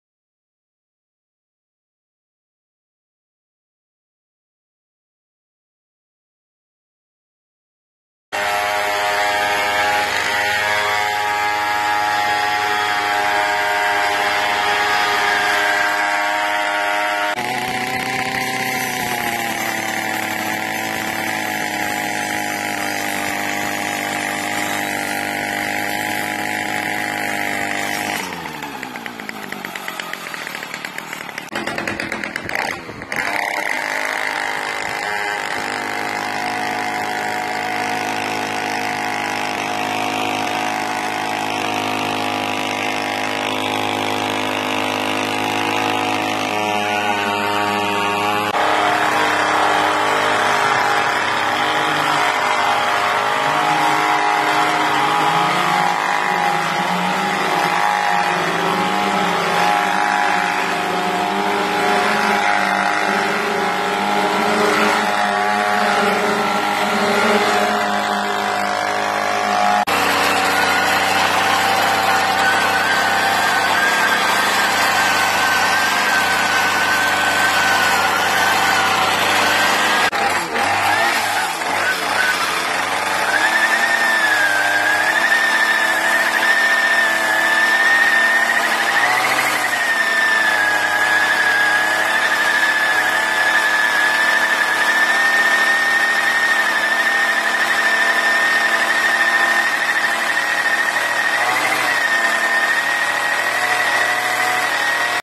Non-standard accessories for brushcutters.
The video presents non-standard accessories for a brush cutter.
Leaf blower and snow removal machine of my idea.
Widia saw for cutting trees
Rice brush for cleaning terraces
And a wire brush

You can buy the wire brush for the scythe